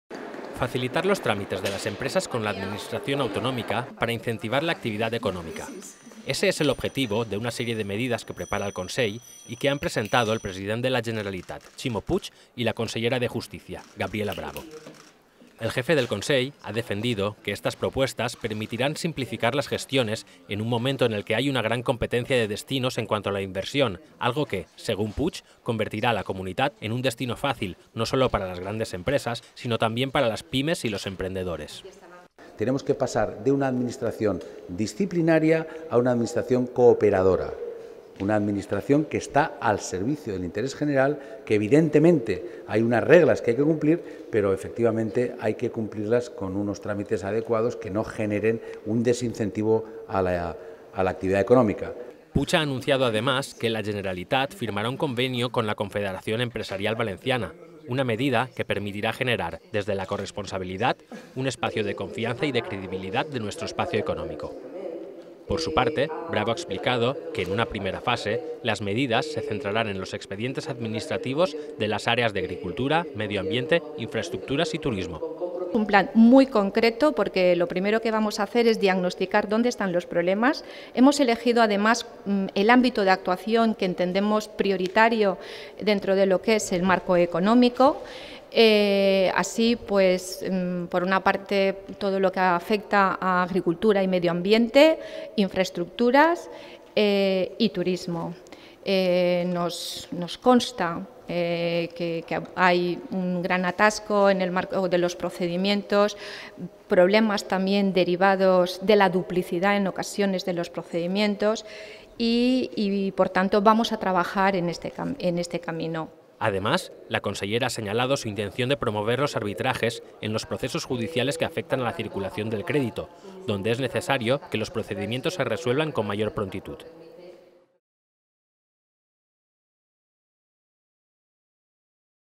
Puig defiende una Administración cooperadora para incentivar la actividad económica
El president de la Generalitat, Ximo Puig, ha defendido el paso de "una Administración disciplinaria a una Administración cooperadora, al servicio del interés general", en la que los trámites administrativos "no generen un desincentivo a la actividad económica". Puig ha presentado, junto a la consellera de Justicia, Administración Pública, Reformas Democráticas y Libertades Públicas, Gabriela Bravo, una propuesta para simplificar las gestiones de las empresas ante la Administración autonómica.

El president de la Generalitat, Ximo Puig, ha defensat el pas de "una Administració disciplinària a una Administració cooperadora, al servei de l'interès general", en la qual els tràmits administratius "no generen un desincentivo a l'activitat econòmica". Puig ha presentat, amb la consellera de Justícia, Administració Pública, Reformes Democràtiques i Llibertats Públiques, Gabriela Bravo, una proposta per a simplificar les gestions de les empreses davant l'Administració autonòmica.